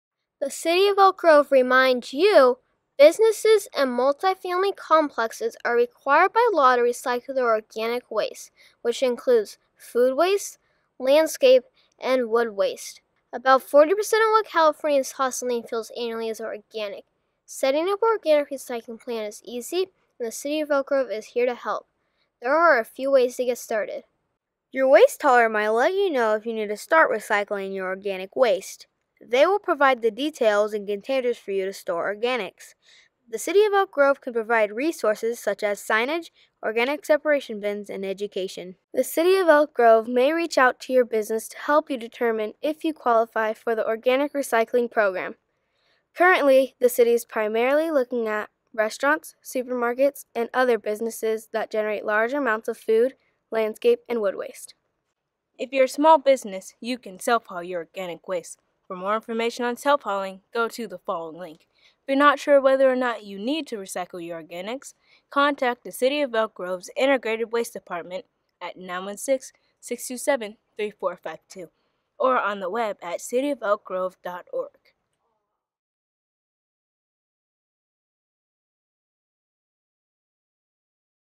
The City of Elk Grove Reminds You How to Set Up Organic Recycling
This film was produced by Curiosity Films students Christopher Kipp, Tristan Landeros, Madisyn Modellas, and Nia Smith for the City of Elk Grove's Integrated Waste Department.

With the passage of California Assembly Bill 1826, organic waste must now be recycled in order to reduce greenhouse gas production and to conserve landfill space. This film is one of a series to educate businesses in our city that excess food does not have to go to the landfill. It can be diverted to people who need it most.

A special thanks to the City of Elk Grove's Integrated Waste Department for the opportunity to serve our community.